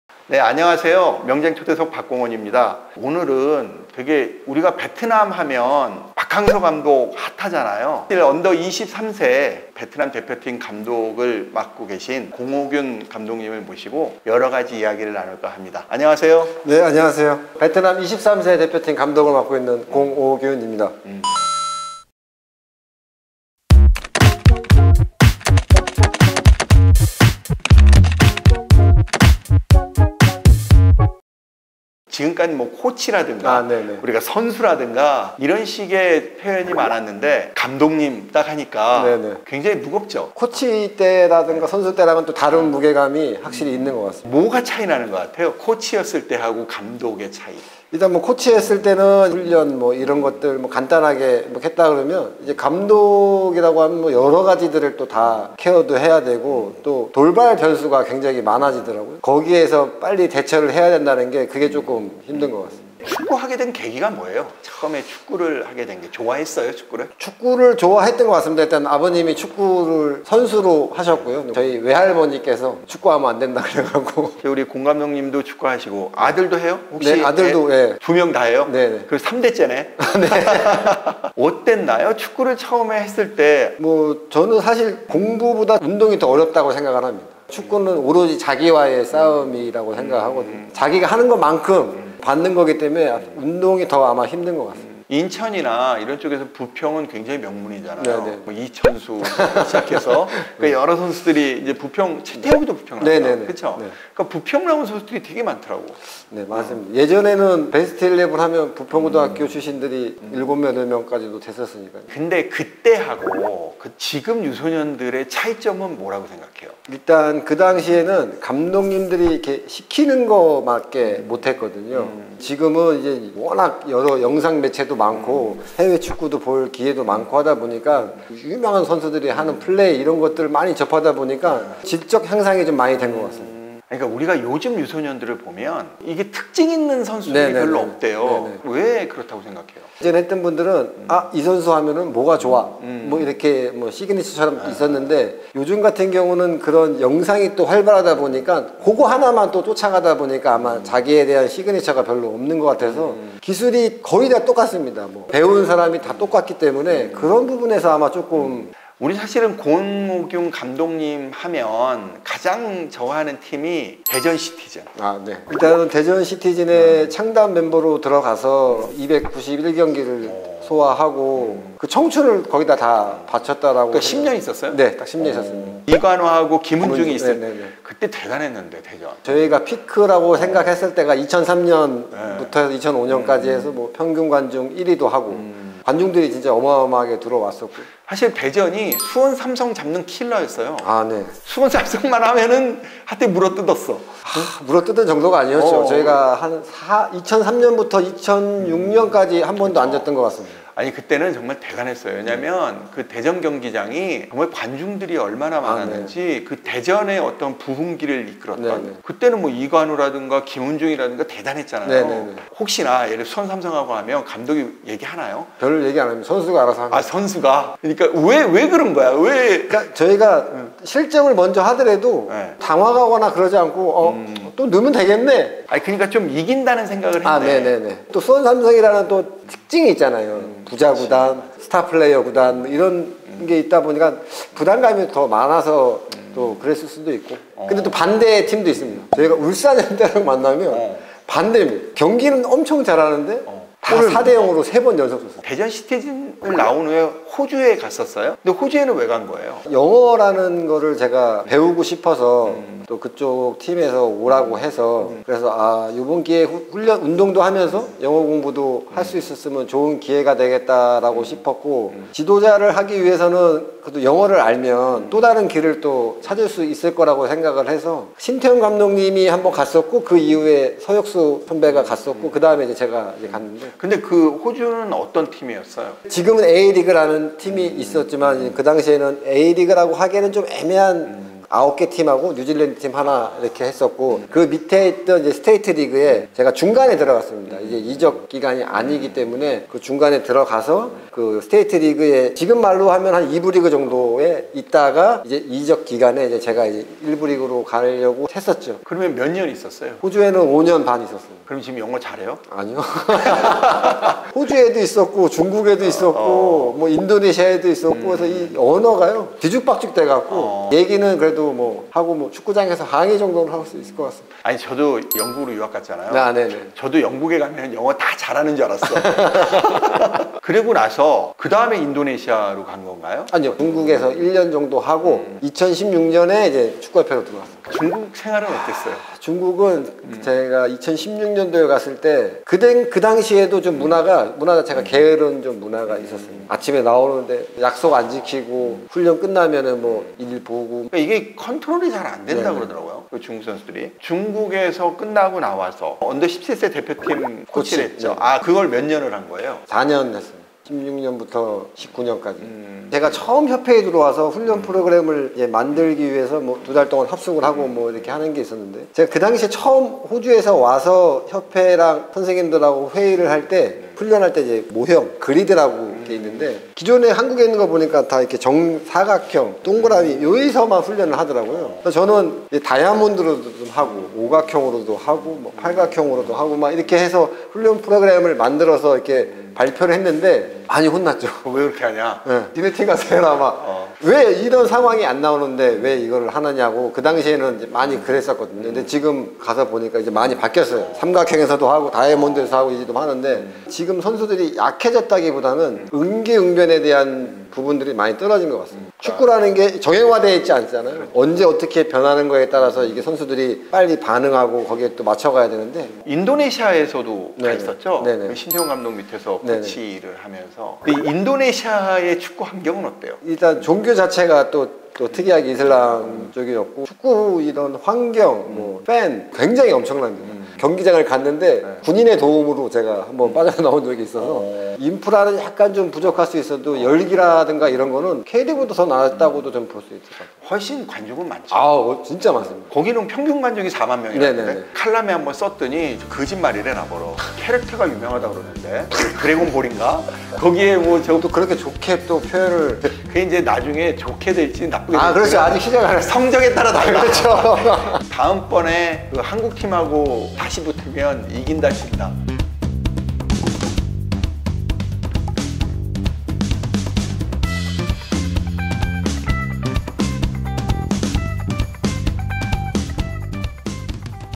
⚽️명장초대석⚽️ 수원삼성 물어뜯던 대전 트리오 공오균!
박항서 감독님의 뒤를 이어 베트남 U-23 감독이자 과거 대전 시티즌에서 맹활약 했던 공오균 감독님을 모시고 이야기 나눠보았습니다.